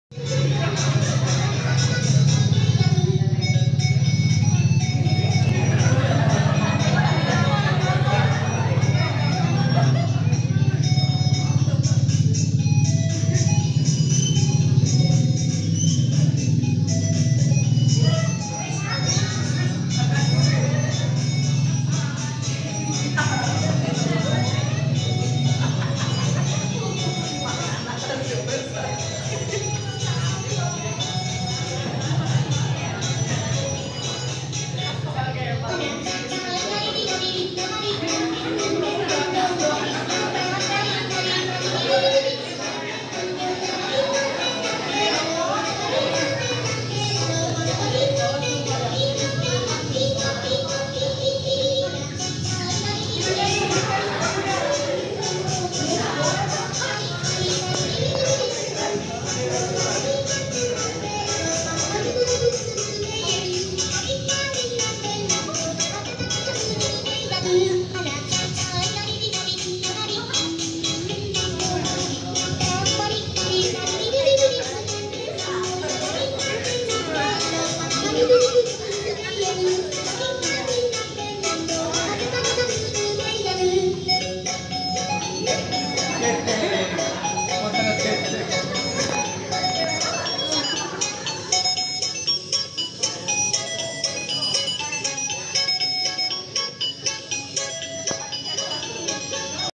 "Joged Penguin"😄 | Rileks Sejenak.. Mengendurkan Otot2 yang Tegang....Wkwk....
Minggu Pagi 24 Juli 2022 di Kompleks Kauman Majenang